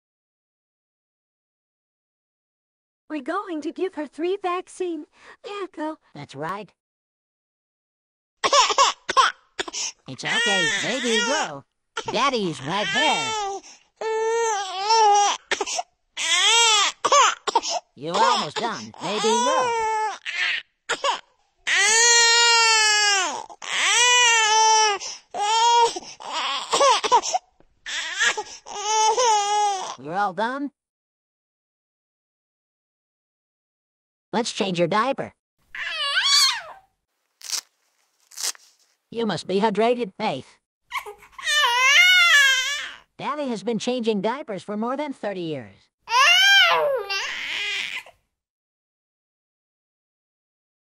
Faith Warner gets 3 vaccines in one (An Animaniacs fanfiction)
Here is a short fanfiction that takes place on August 5, 2022. Yakko is comforting his 6 month old baby girl, Faith, while she gets her vaccines.

Faith turned 6 months old on July 29, 2022, and she is the 5th born child of Yakko Warner and Laffy Warner (OC by Laffimaniac).